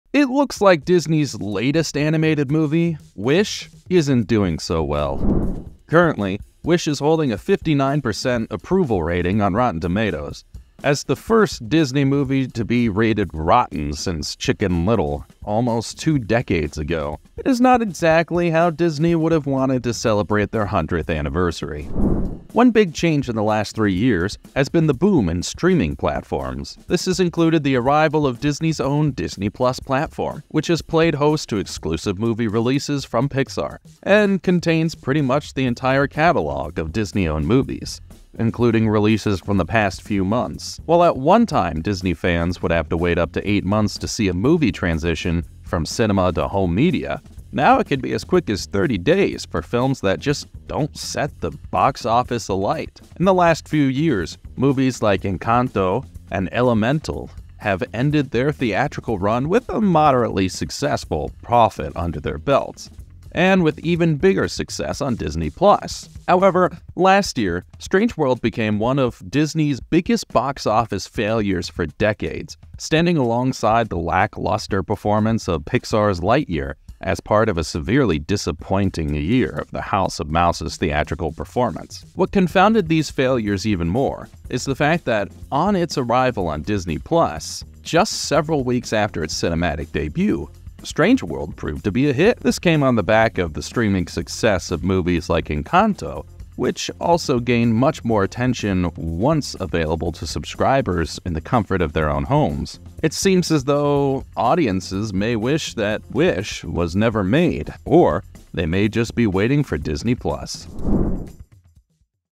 Disney’s Wish Unfulfilled With Rotten Tomatoes Score Worse Than Last Year's Box Office Disaster
Back in 2019, Disney seemed to be riding high on a wave of success. From the mighty Avengers franchise under their Marvel Studios banner, through the animation of Pixar and Walt Disney Animation, to the live-action remakes of Beauty and the Beast, The Lion King and Aladdin, it seemed that little could stop the House of Mouse’s dominance of the box office. However, Disney’s fortunes have taken a turn for the worse in recent years, and it looks like their latest animated movie, Wish, is going to have its own struggle to turn things around after a poor initial wave of reviews.

00:10 Start
00:25 Disney Pllus
01:00 Elemental
1:33 Strange World